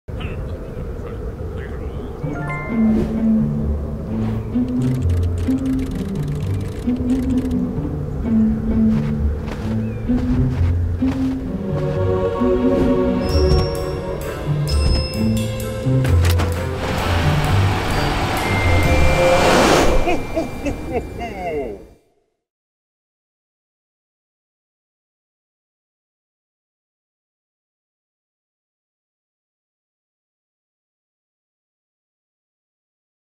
Happy Holidays - Babbo Natale usa Mercedes-Benz Unimog.
La renna Rudolph è indisponibile. Per fortuna c'e' Unimog di Mercedes-Benz.